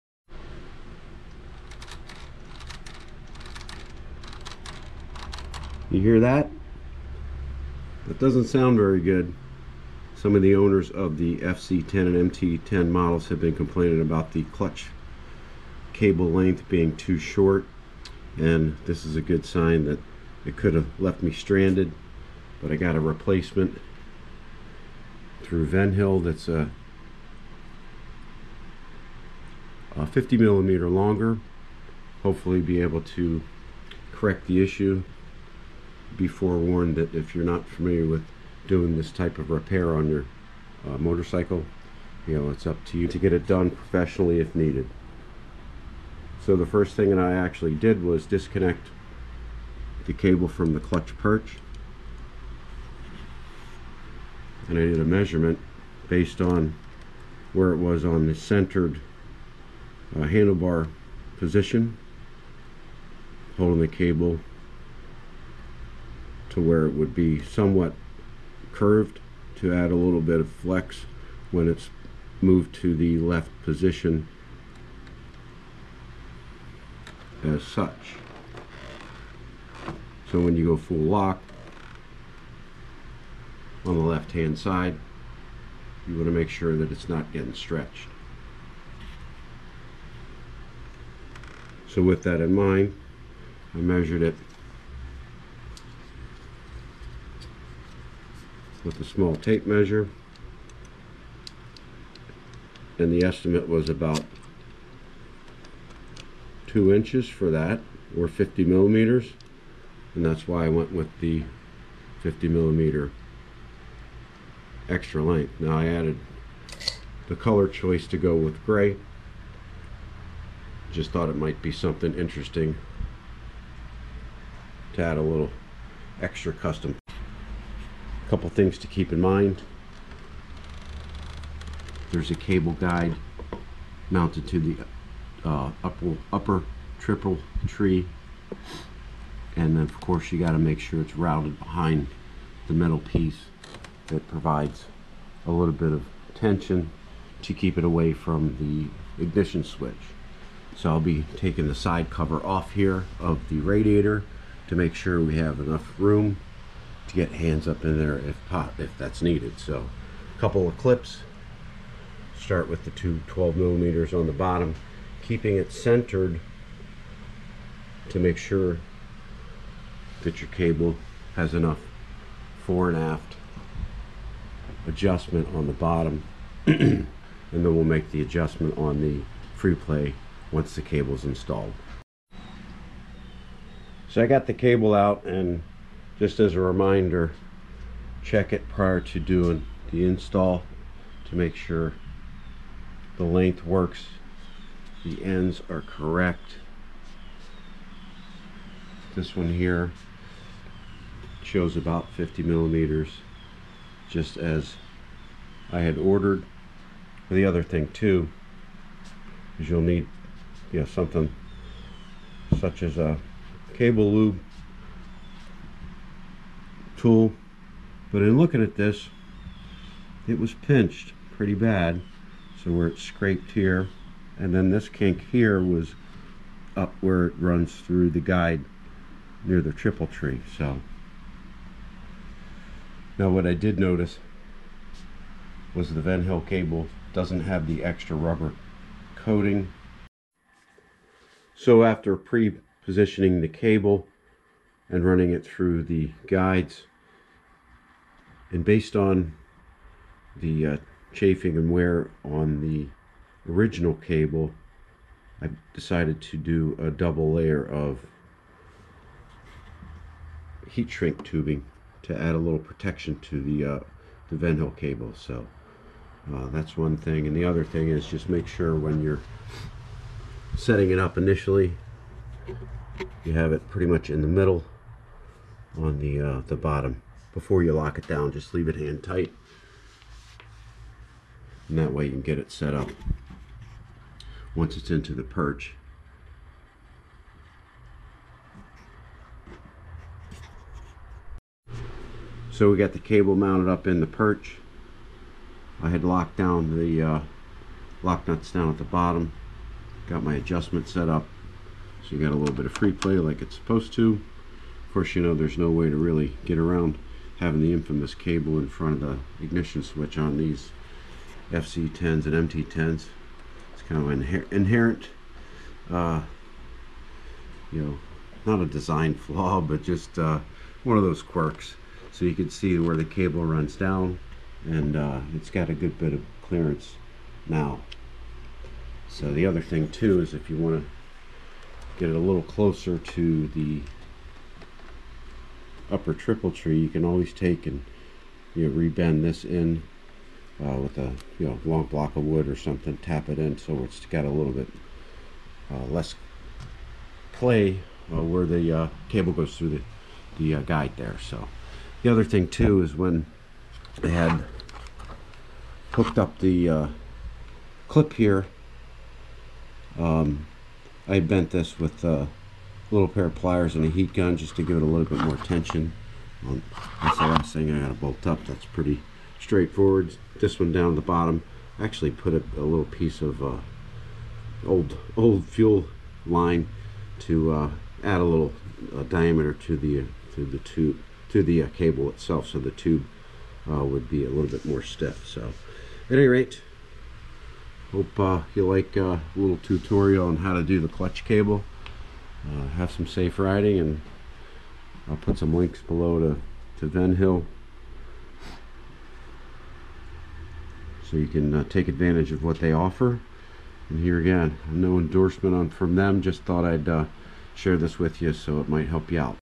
Yamaha FZ10 / MT10 - Upgrade your clutch cable that's too short.
Overview of the clutch cable replacement I did to correct the short cable issue I had on my FZ10.